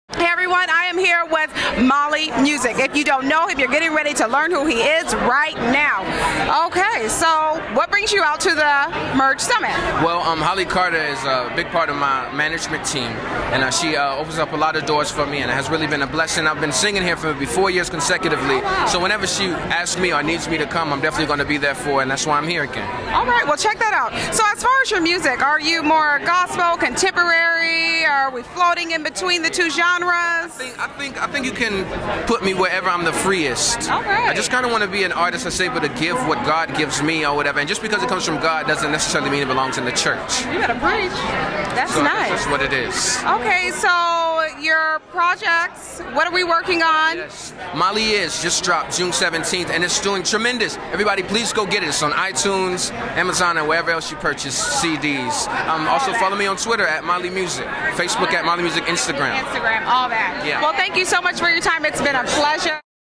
Mali Music @ 2014 Merge Summit Red Carpet | Black Hollywood Live Interview
Black Hollywood Live host Corrinne Williams talks with musician Mali Music on the red carpet at the opening night of the 2014 Merge Summit in Downtown Los Angeles.